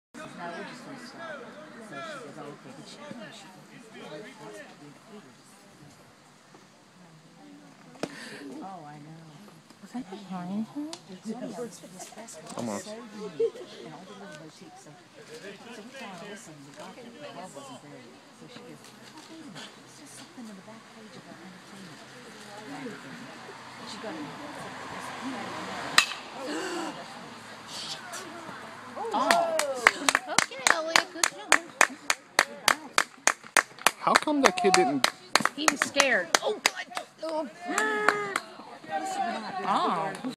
Elliot Mast 2013 Altoona Area High School Fall Ball Bases Loaded Triple
Altoona Area High School Fall Ball Team Vs. Penns Valley - Bases Loaded Triple. Age 15.